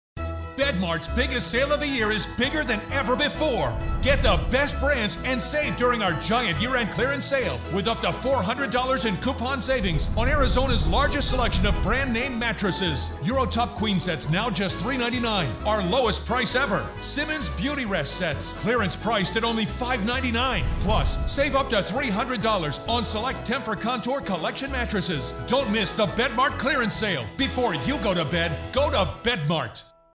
Bedmart end of the year sale
Bedmart's biggest sale of the year is bigger than ever before!  Get the best brands and save during our giant year end clearance sale with up to $400 off in coupon savings on Arizona's largest selection of brand name mattresses.

Save on Simmons, Eurotops, Tempurpedic and more!

Before you go to bed, go to Bedmart!